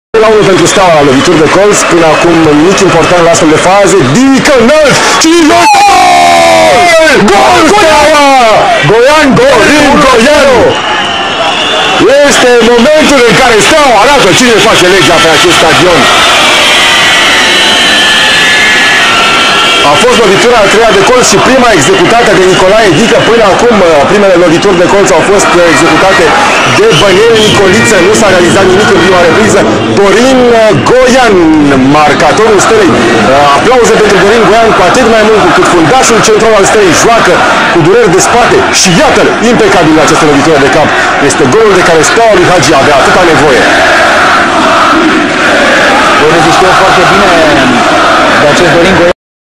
UEFA Champions League 2007/2008 Turul 2 Zaglebie Lubin vs Steaua Bucuresti 0 -1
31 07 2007
UEFA Champions League 2007/2008 Turul 2